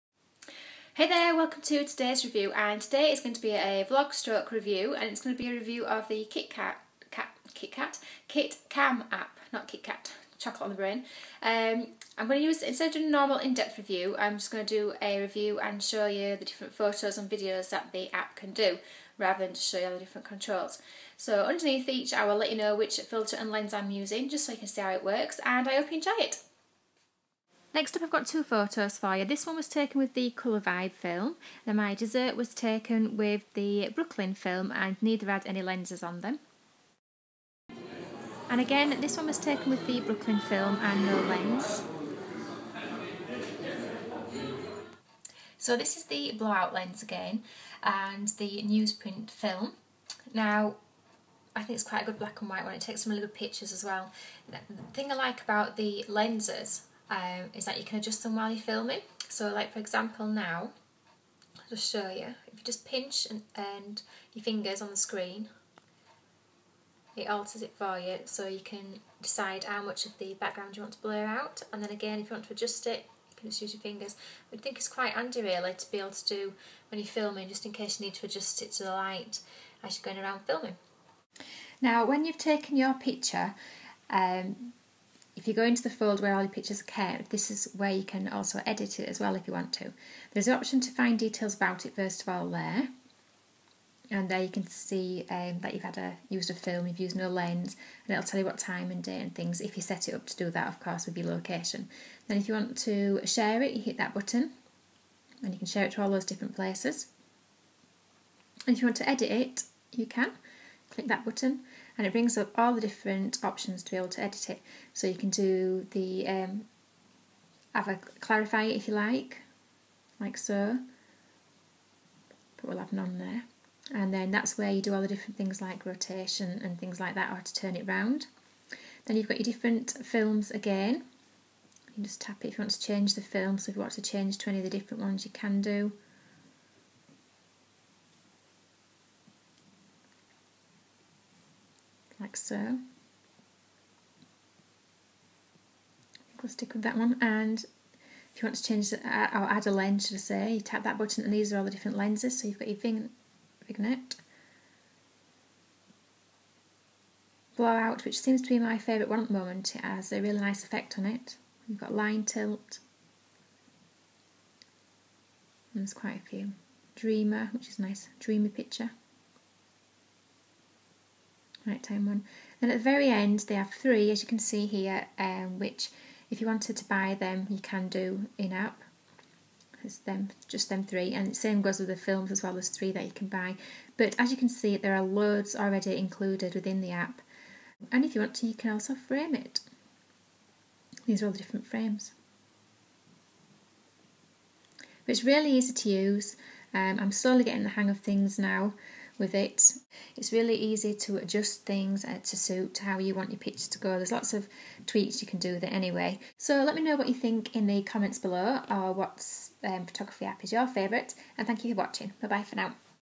Vlog/Review using the KitCam iPhone app
A blog style review of the KitCam app. This is a short brief review showing you the films and lens options. Just so you get an idea of how it works.